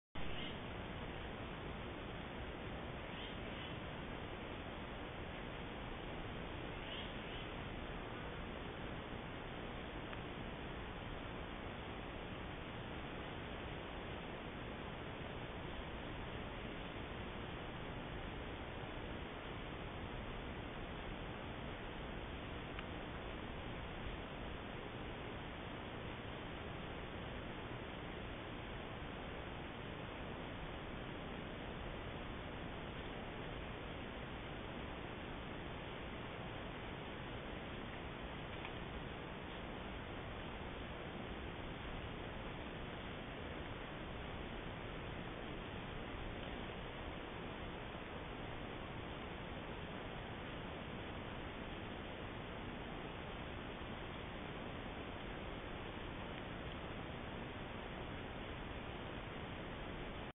Minerals & Whitetails 6
A Moore County, NC Buck we call G2 making use of a mineral station that was created in March 2016